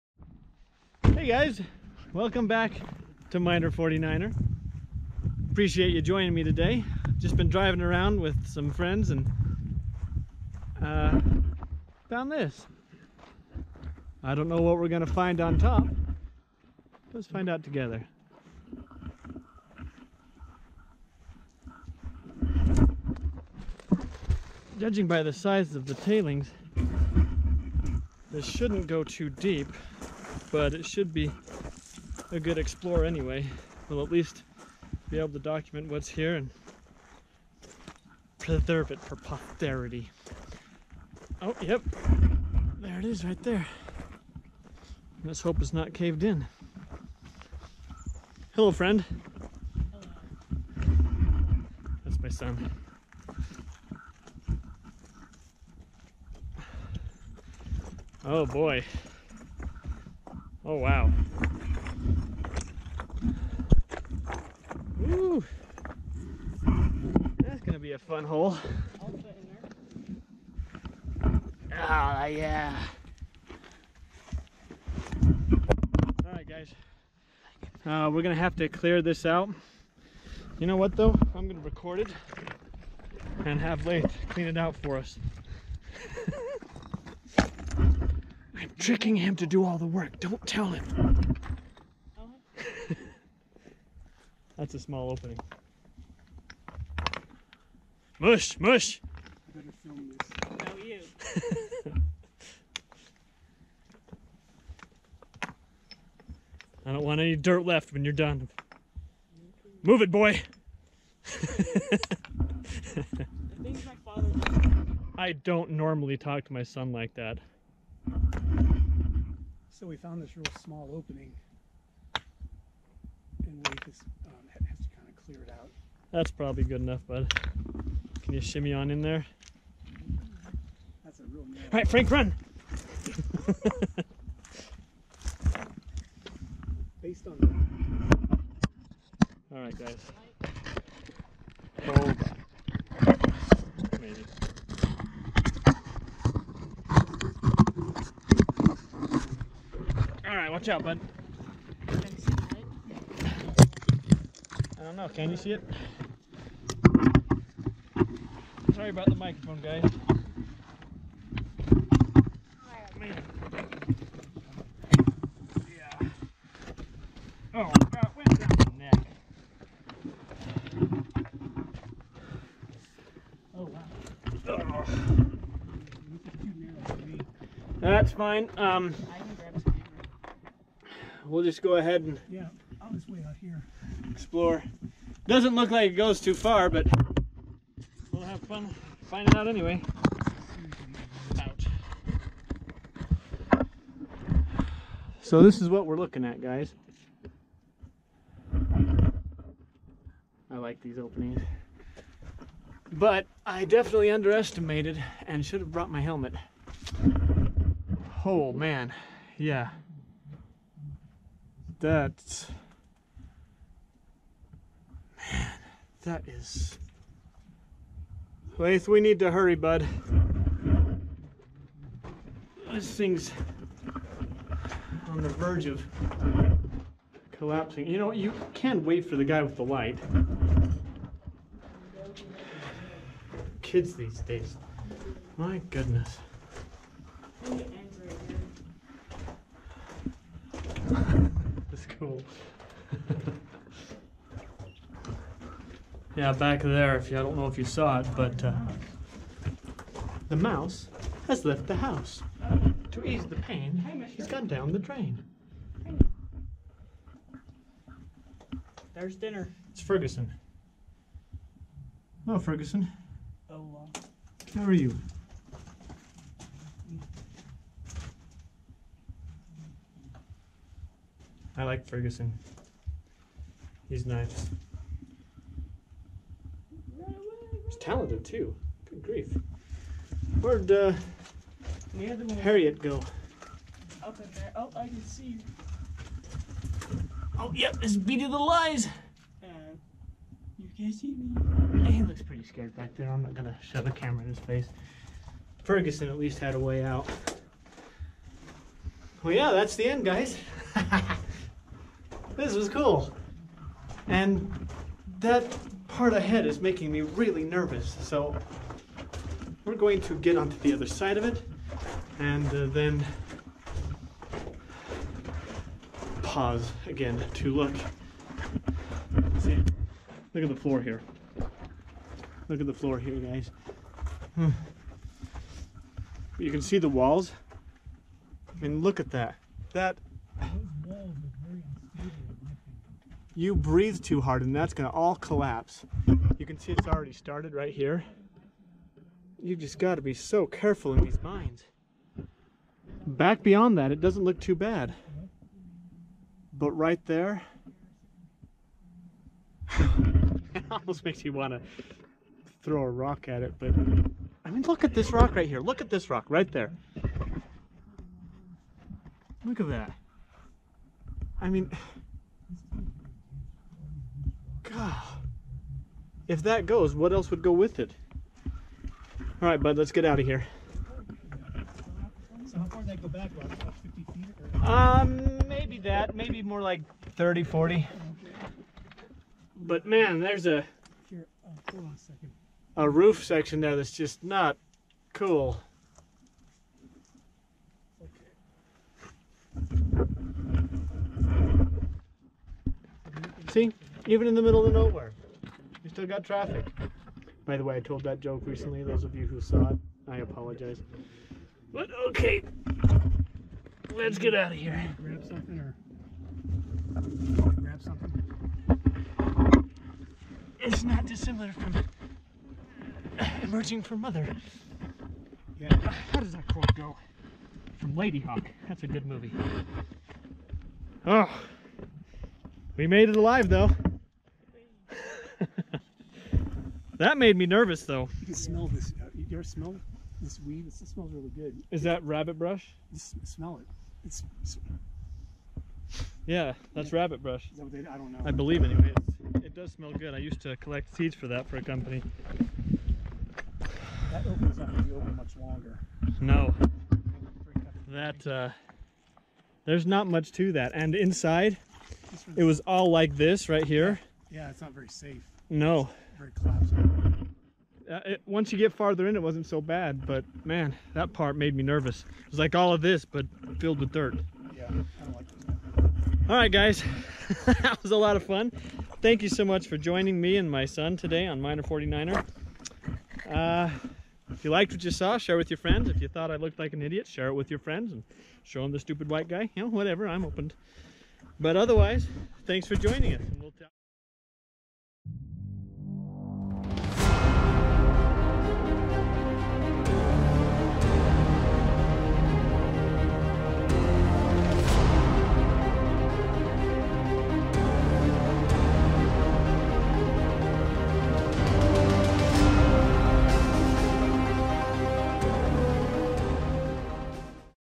Dangerous Little Hole in the Ground
Fun little mine with a friendly surprise inside...once you pass the crumbling roof! Thanks for joining me, and I hope you enjoy this dangerous little hole in the ground!